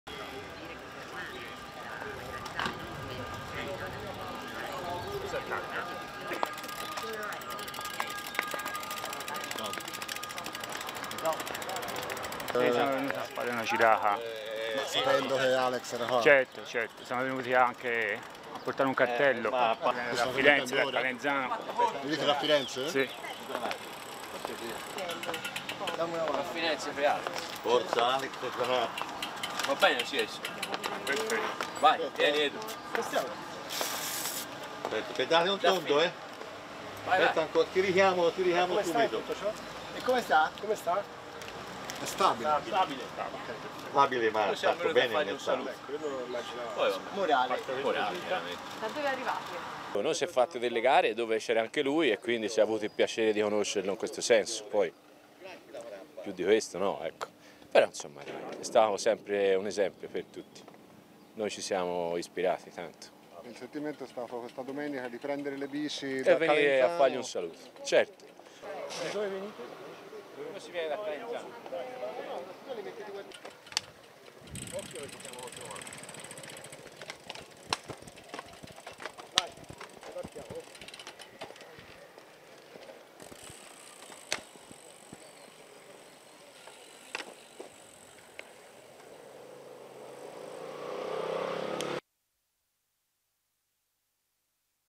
Alex Zanardi, in bici da Firenze a Siena per rendere omaggio al campione
Cinque ciclisti, partiti da Firenze, sono arrivati davanti all'ospedale Santa Maria alle Scotte di Siena per incoraggiare Alex Zanardi, rimasto vittima di un incidente con la sua handbike a Pienza venerdì scorso. "Siamo qui per portare la nostra solidarietà ad Alex. Facciamo parte di una squadra di triathlon di Calenzano (Firenze) e avendo anche gareggiato con lui, ci è sembrato giusto venire qui per incoraggiarlo". di Giulio Schoen